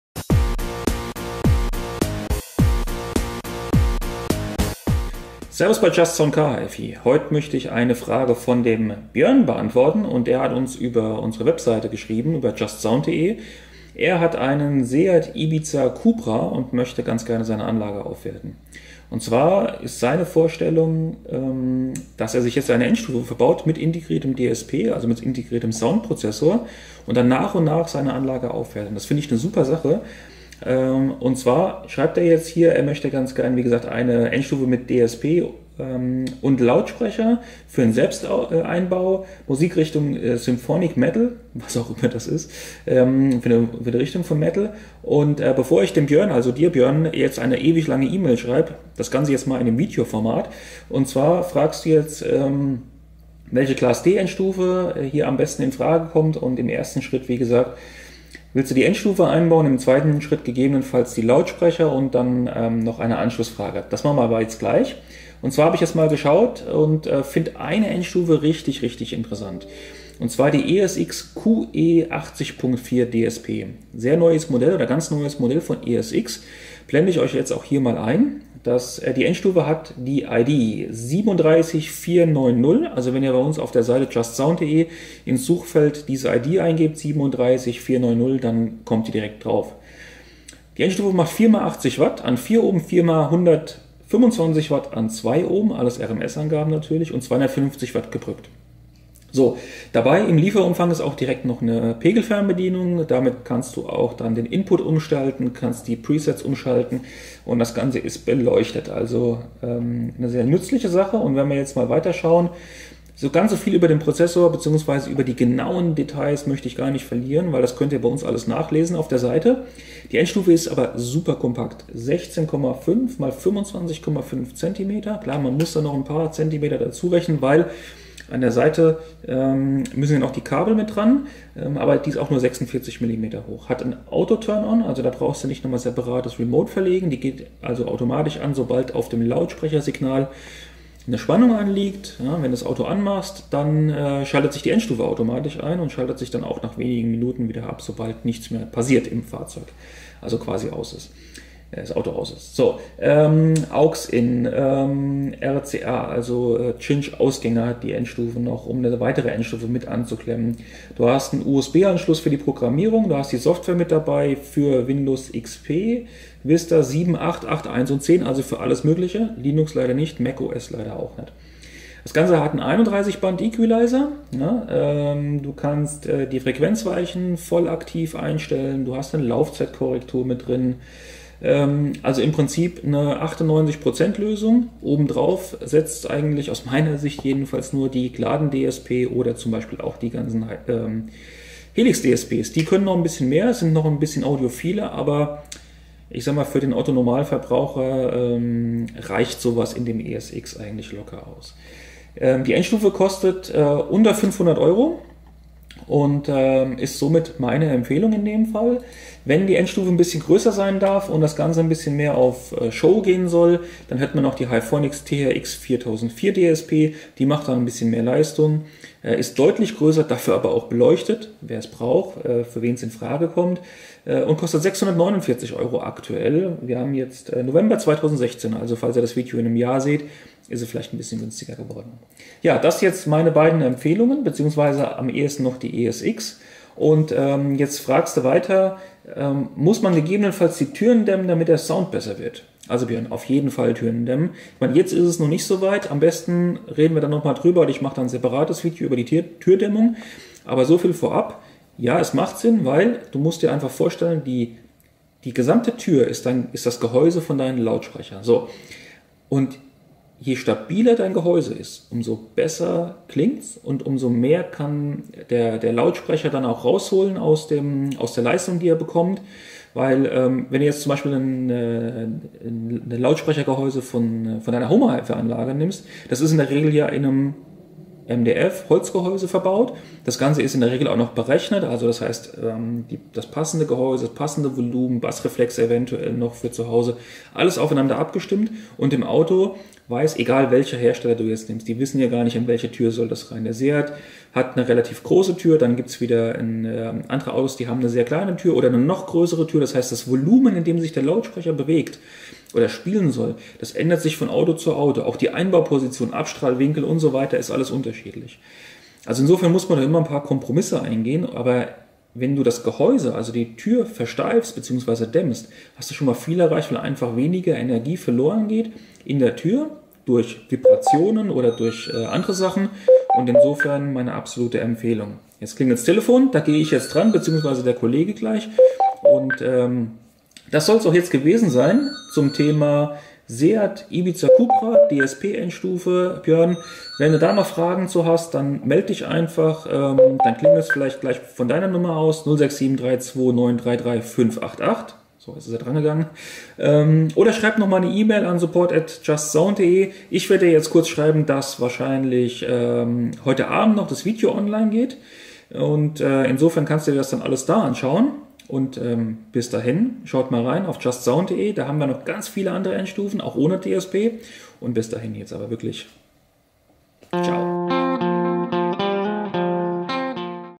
Welche Class-D DSP Endstufe bis 700€? ESX QE80.4 - just-SOUND CarHifi Onlineshop
Wir beantworten hier die Frage von Björn nach einer sehr kompakten DSP Endstufe für sein Seat Ibiza Cupra von 2012

Schaut mal rein und viel Spaß

Euer just-SOUND CarHifi Team